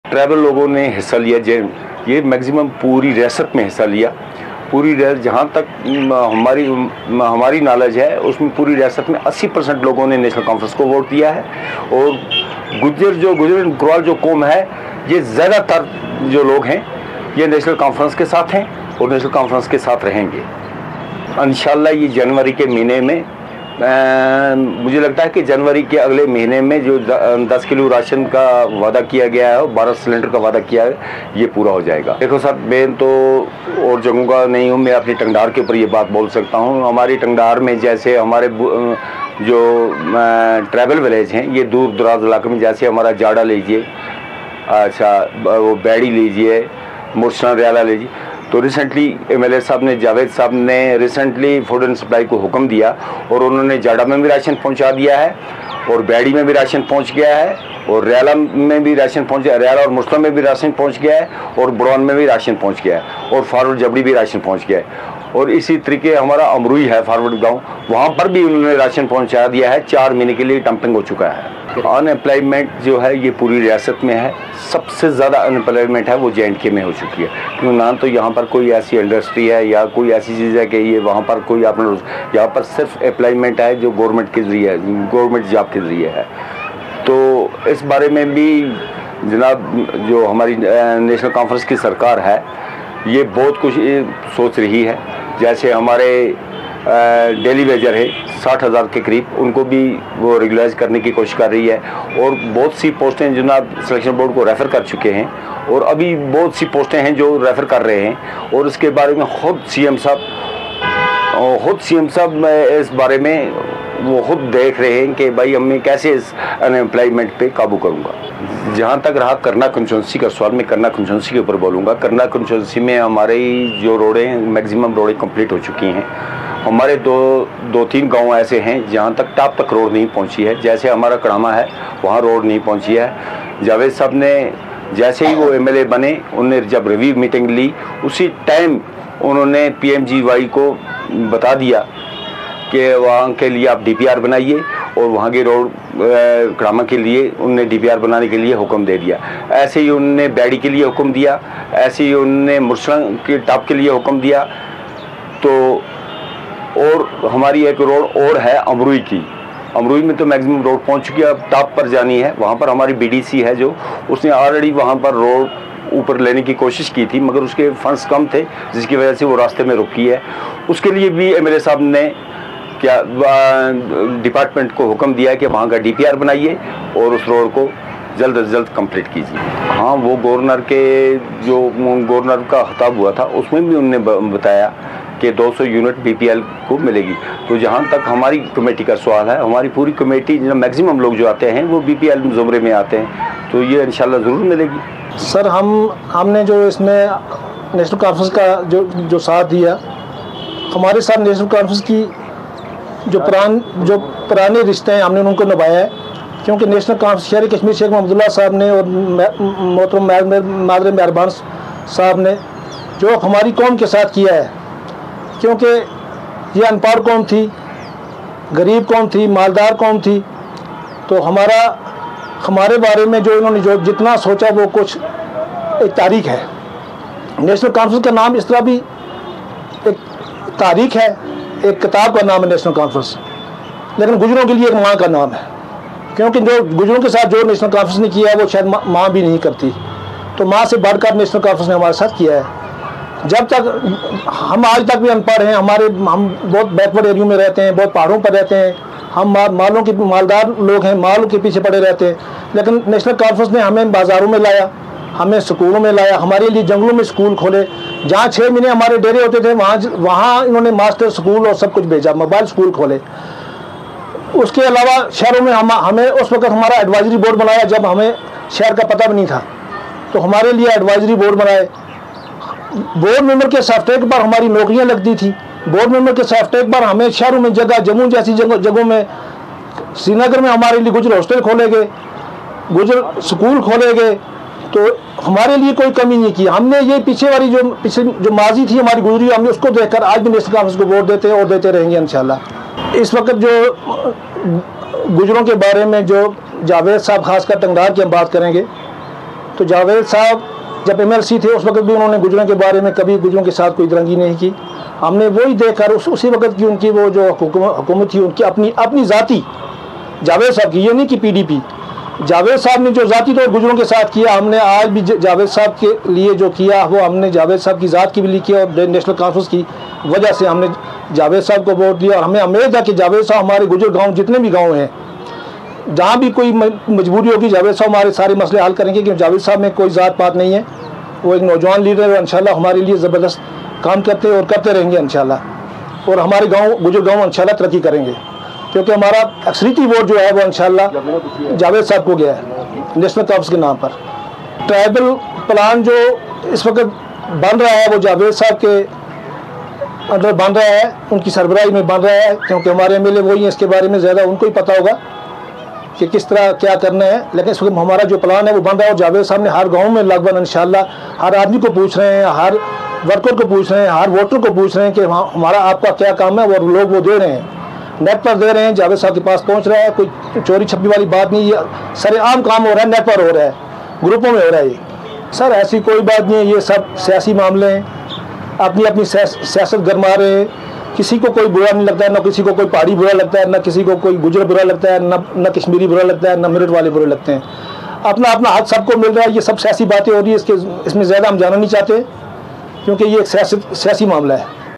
Former Sarpanch Chowdhary Mohammad Ashraf and senior Gujjar-Bakarwal leader Mohammad Ashraf Paswal
Former Sarpanch Chowdhary Mohammad Ashraf and senior Gujjar-Bakarwal leader Mohammad Ashraf Paswal address the media, emphasizing the tribal development plan, uplifting the tribal community, and  unemployment issues